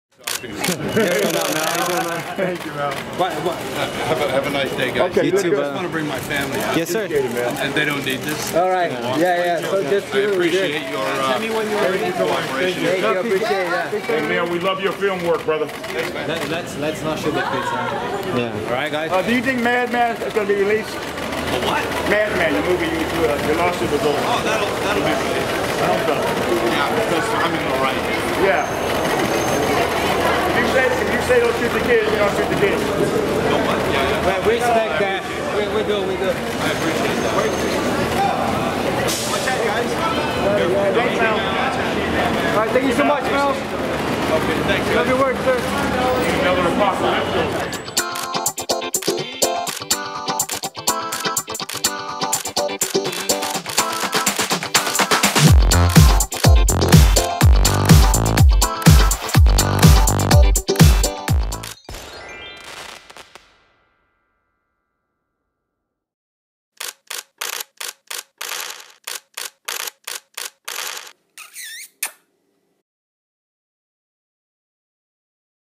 Mel Gibson Best Mood Ever? Chatting at LAX!
Mel Gibson Best Mood Ever? Seen Chatting at LAX!

Who. Mel Gibson

What. Being in a good mood

Wearing. Baseball cap and sunglasses

Where? 1 World Way, Los Angeles, CA 90045, USA

The Braveheart Oscar Winner was spotted arriving back from Vacation at Los Angeles International Airport and appeared to be in the best mood ever as he chatted with photographers waiting to take his picture!

Mel flashed a huge smile as he pushed his own luggage to the waiting car as his family followed behind him

You can also visit our Celebrity WotNot website for more great celebrity fun: